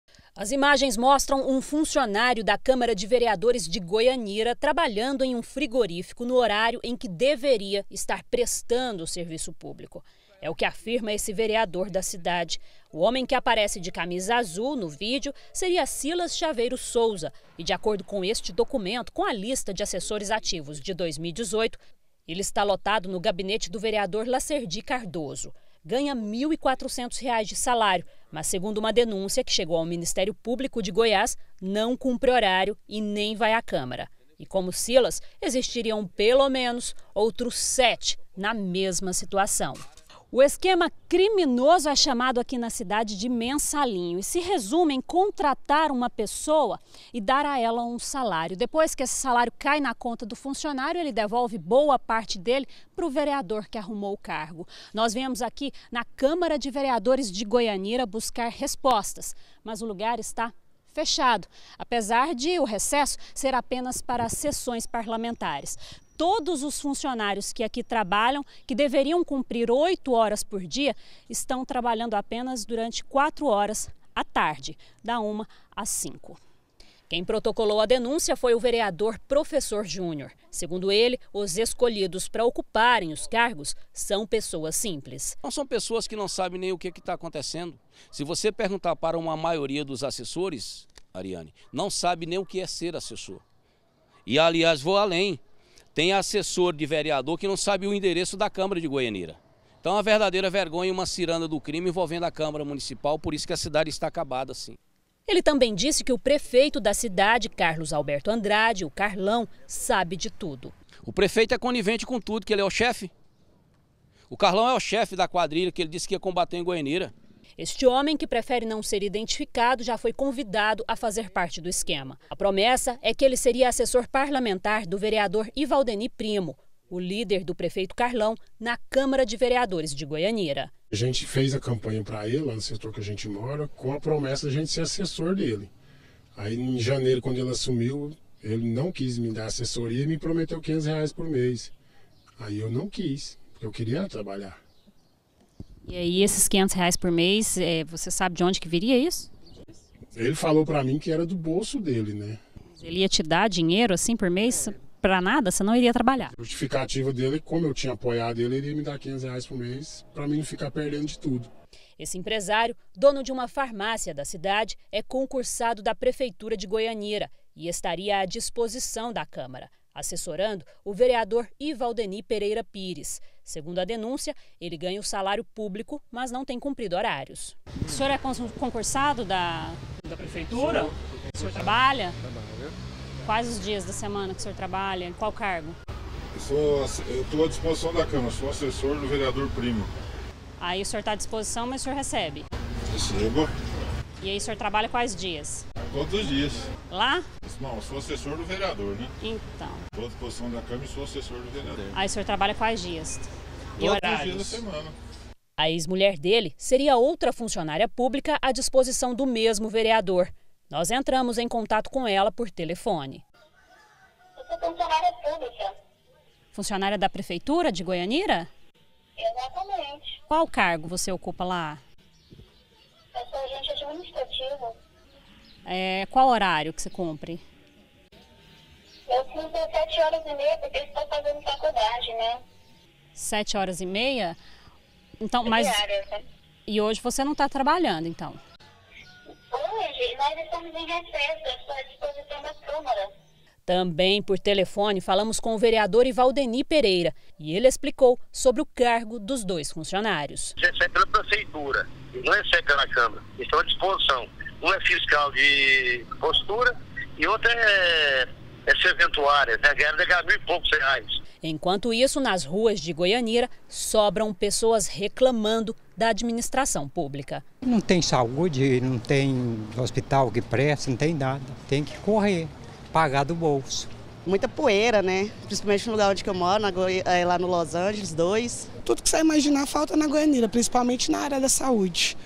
BG - Denúncia em Goianira: funcionários fantasmas na câmara e na prefeitura - 27-07-2018
Vereadores de Goianira denunciam que alguns funcionários da câmara municipal e da prefeitura estariam recebendo sem trabalhar. Os moradores estão revoltados.